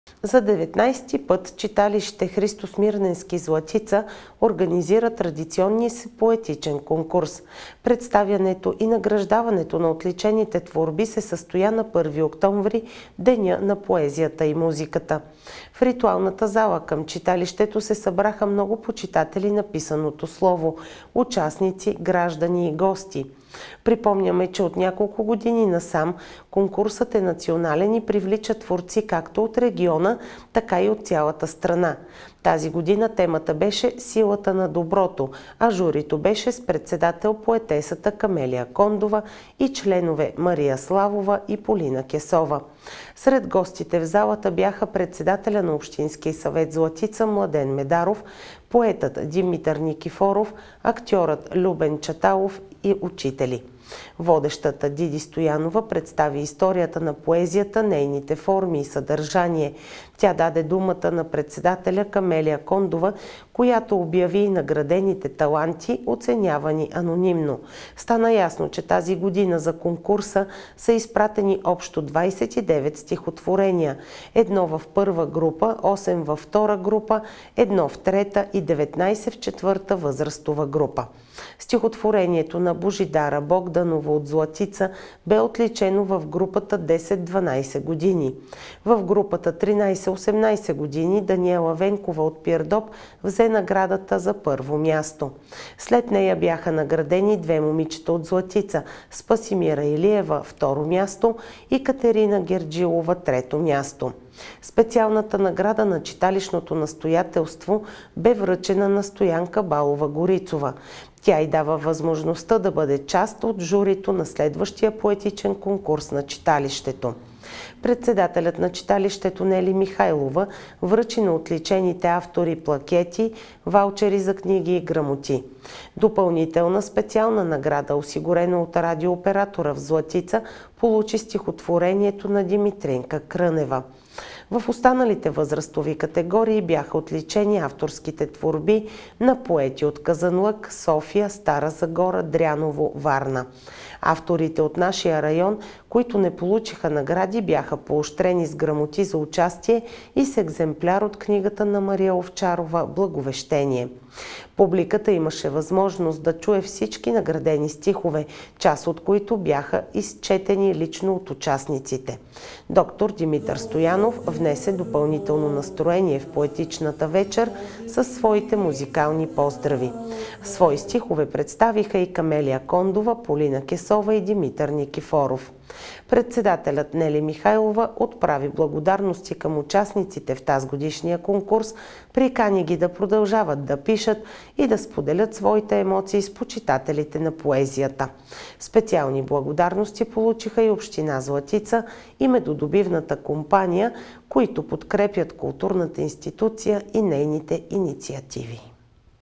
В Златица проведоха 19-и поетичен конкурс
За деветнадесети път читалище „Христо Смирненски“ Златица организира традиционния си поетичен конкурс. Представянето и награждаването на отличените творби се състоя на 1 октомври -  Деня на поезията и музиката.
В Ритуалната зала към читалището се събраха много почитатели на писаното слово - участници, граждани и гости. Припомняме, че от няколко години насам конкурсът е национален и привлича творци, както от региона, така и от цялата страна.
Тази година темата беше „Силата на доброто“, а журито беше с председател поетесата Камелия Кондова и членове Мария Славова и Полина Кесова. Сред гостите в залата бяха председателят на Общински съвет - Златица Младен Медаров, поетът Димитър Никифоров, актьорът Любен Чаталов, учители.
Водещата Диди Стоянова представи историята на поезията, нейните форми и съдържание. Тя даде думата на председателя Камелия Кондова, която обяви и наградените таланти, оценявани анонимно. Стана ясно, че тази година за конкурса са изпратени общо 29 стихотворения – едно в първа група, осем във втора група, едно в трета и 19 в четвърта възрастова група.
Стихотворението на Божидара Богданова от Златица бе отличено в групата 10-12 години. В групата 13-18 година Даниела Венкова от Пирдоп взе наградата за първо място. След нея бяха наградени две момичета от Златица - Спасимира Илиева, второ място, и Катерина Герджилова, трето място. Специалната награда на читалищното настоятелство бе връчена на Стоянка Балова – Горицова. Тя й дава възможността да бъде част от журито на следващия поетичен конкурс на читалището.
Председателят на читалището Нели Михайлова връчи на отличените автори плакети, ваучери за книги и грамоти.
Допълнителна специална награда, осигурена от радиооператора в Златица, получи стихотворението на Димитринка Крънева.
В останалите възрастови категории бяха отличени авторските творби на поети от Казанлък, София, Стара Загора, Дряново, Варна.
Авторите от нашия район, които не получиха награди, бяха поощрени с грамоти за участие и с екземпляр от книгата на Мария Овчарова „Благовещение“.
Публиката имаше възможност да чуе всички наградени стихове, част от които бяха изчетени лично от участниците. Д-р Димитър Стоянов внесе допълнително настроение в поетичната вечер със своите музикални поздрави. Свои стихове представиха и Камелия Кондова, Полина Кесова и Димитър Никифоров.
Председателят Нели Михайлова отправи благодарности към участниците в тазгодишния конкурс, прикани ги да продължават да пишат и да споделят своите емоции с почитателите на поезията. Специални благодарности получиха и община Златица и медодобивната компанията, които подкрепят културната институция и нейните инициативи.